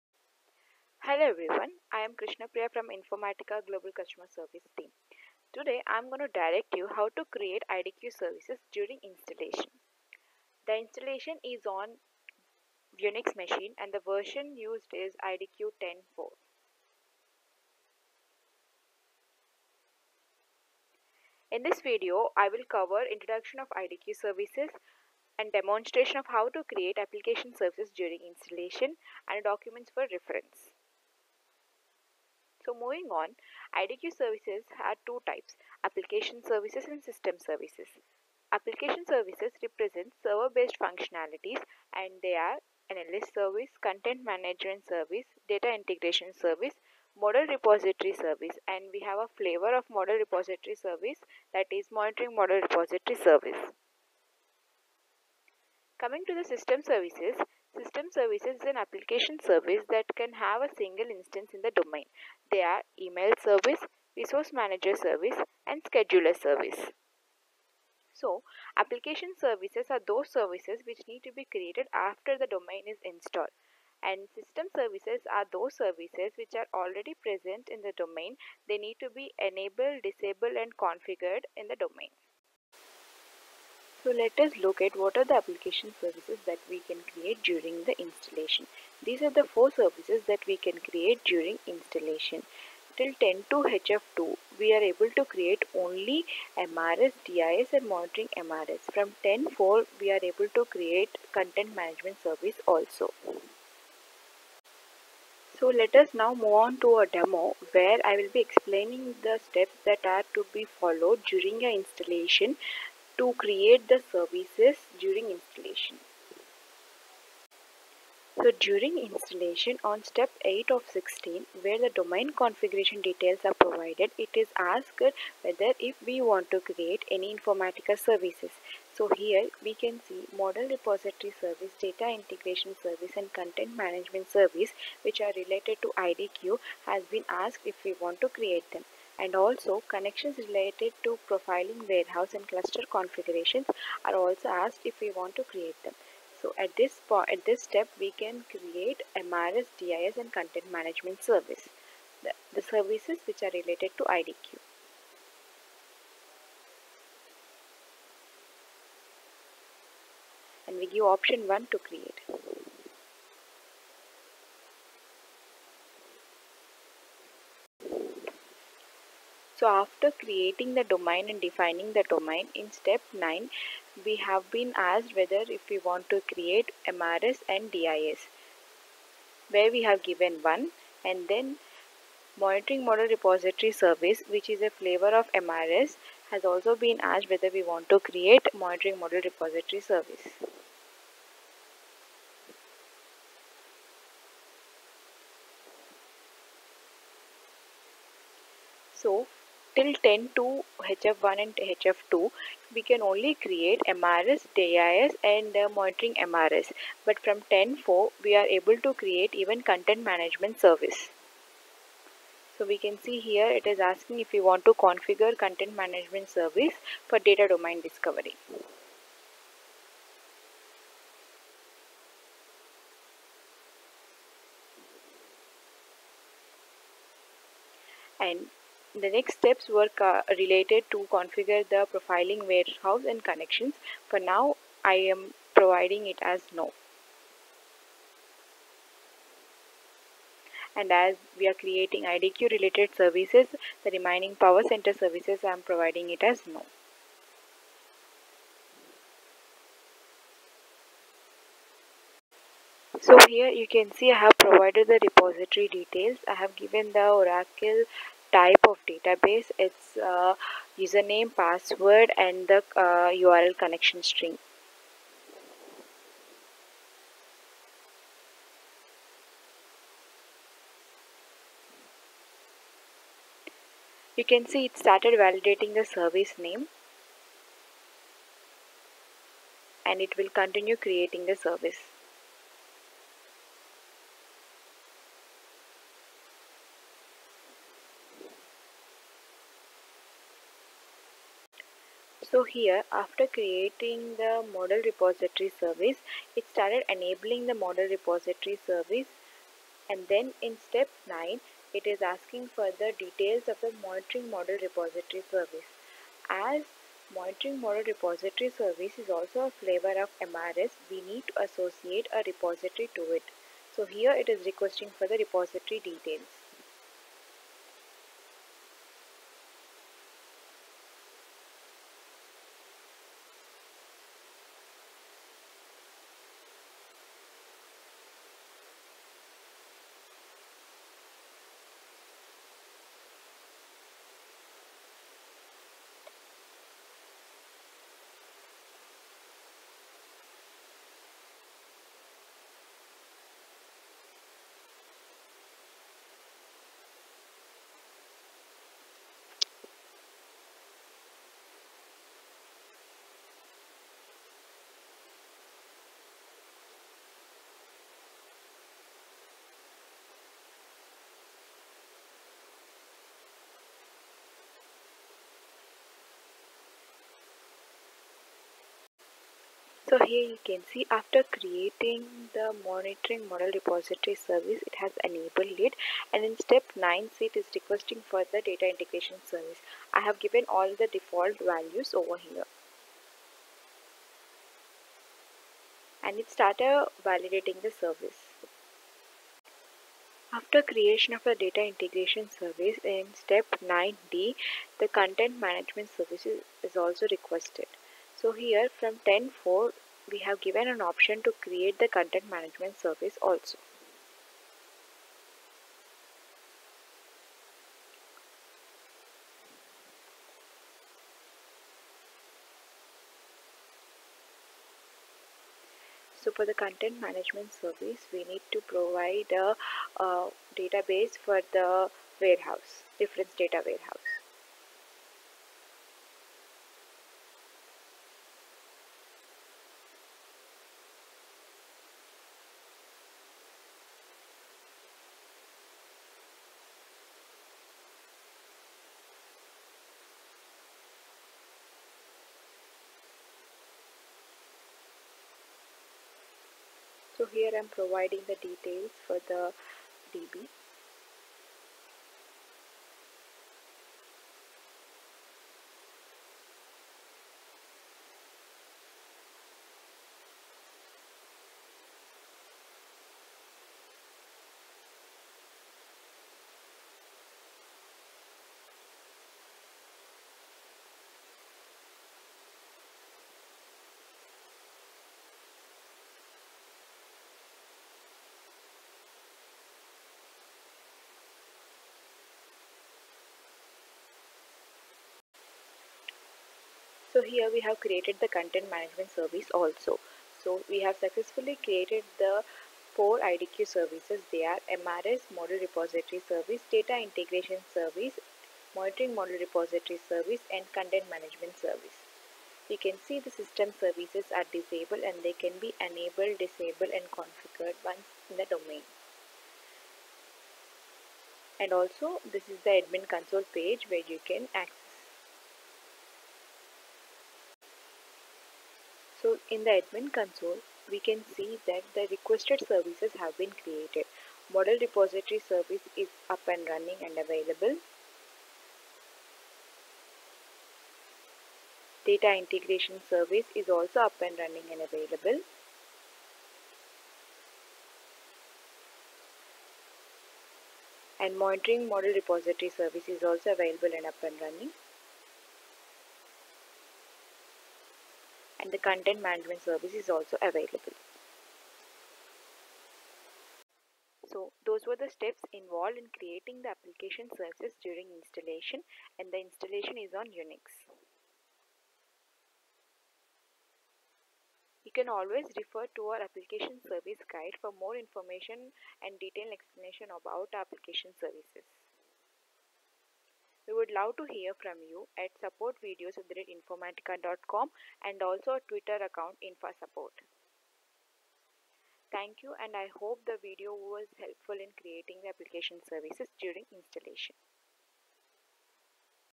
How to Create IDQ Services during Installation (UNIX)
This video explains how to created IDQ Services during installation (UNIX).

Expertise: Beginner
User Type: Administrator
Category: Configuration/Installation
Project Phase: Configure
Problem Type: Configuration/Installation